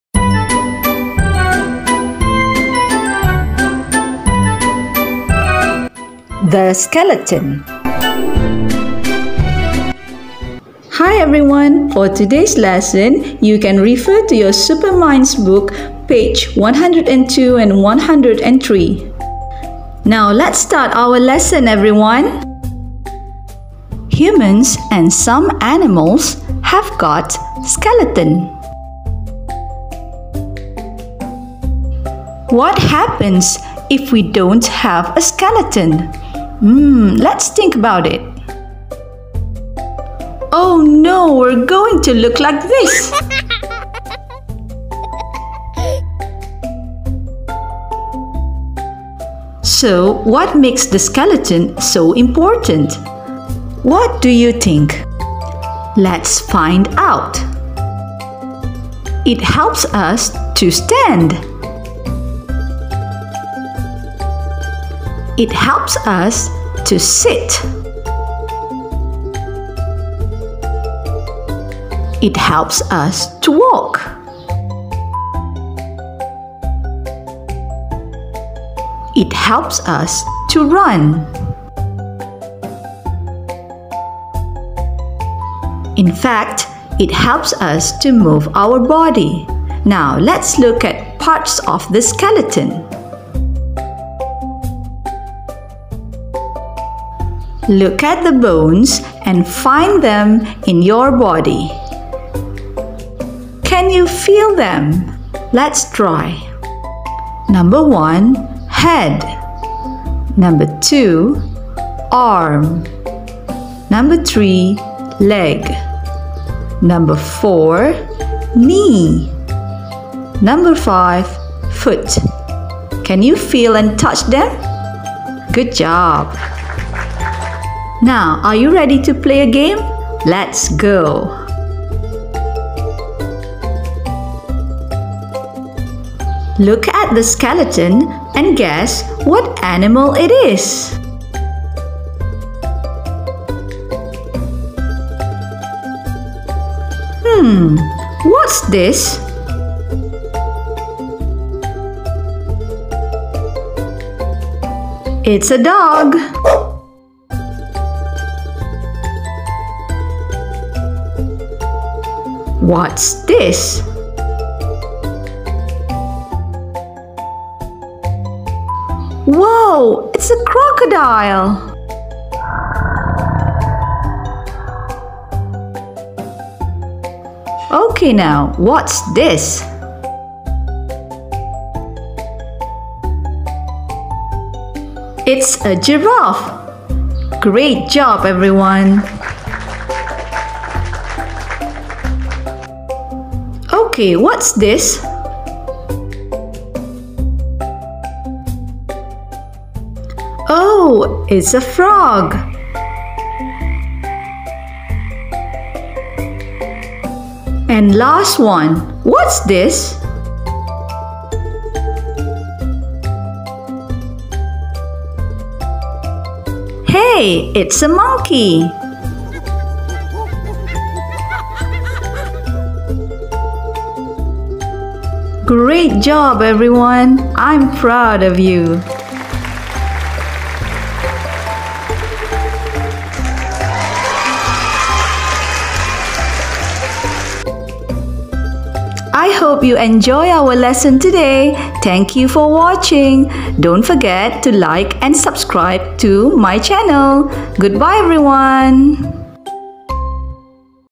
CEFR ENGLISH YEAR 2: THE SKELETON
Learn about parts of the skeleton and its importance to our bodies.